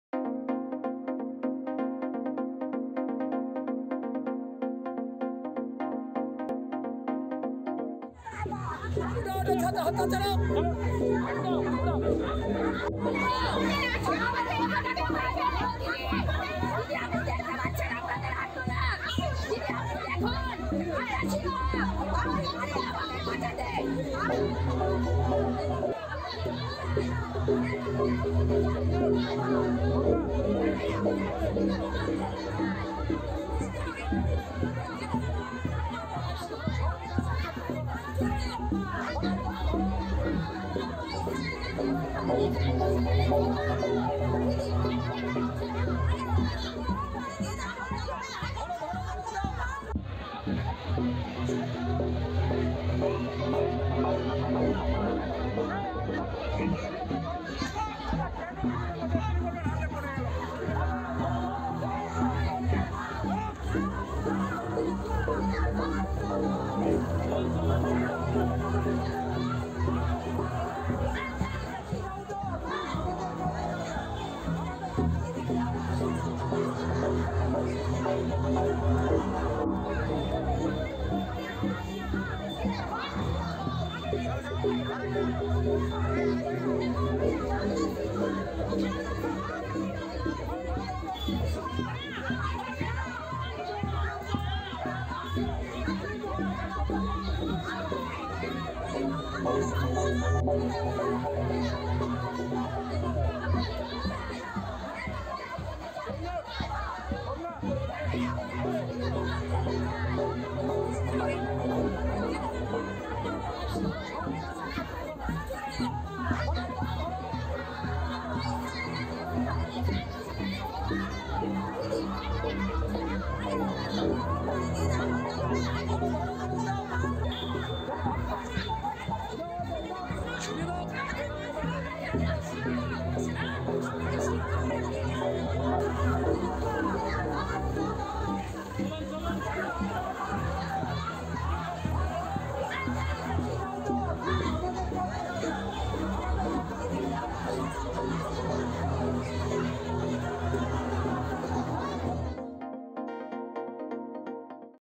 একাধিক দাবিতে নবান্নের সামনে বিক্ষোভ শিক্ষক ঐক্য মঞ্চের
বেতন বৃদ্ধি-সহ একাধিক দাবিতে নবান্নের সামনে বিক্ষোভ শিক্ষক ঐক্য মঞ্চের। সেই বিক্ষোভ কর্মসূচি ঘিরে ধুন্ধুমার বাঁধল নবান্নের সামনে। দুপুর ২ টোর কিছুটা পরেই ২৫-৩০ জন শিক্ষক ও অশিক্ষককর্মী নবান্নের সামনে চলে আসেন। বেতন কাঠামো সংশোধন-সহ একাধিক দাবি জানাতে থাকেন। বিস্তারিত দেখুন ভিডিয়োয়